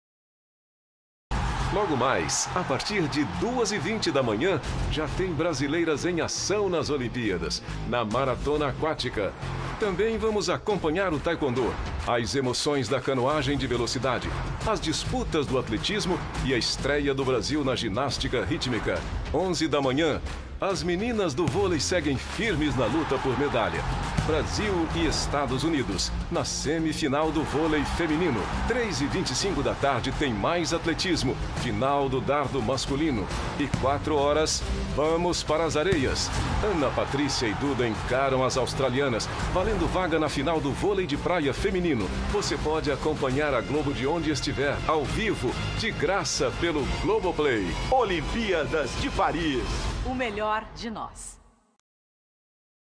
Paris 2024: dia 13 - chamada (8 Ago)
Chamada com a programação prevista do 13º dia de JogosOlímpicos Paris2024.
Com maratona aquática, canoagem de velocidade, atletismo, ginástica rítmica, vôlei, vôlei de praia, entre outros.

NO AR:
8 Agosto 2024 | Globo